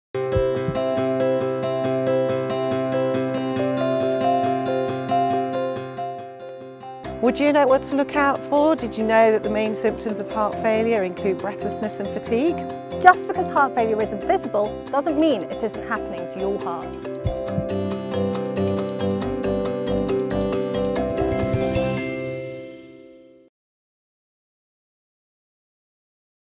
What Are the Main Symptoms of Heart Failure ? - Nurse Louise Clayton & Resham Baruah
Discover important symptoms of heart failure from heart failure clinicians in this short video.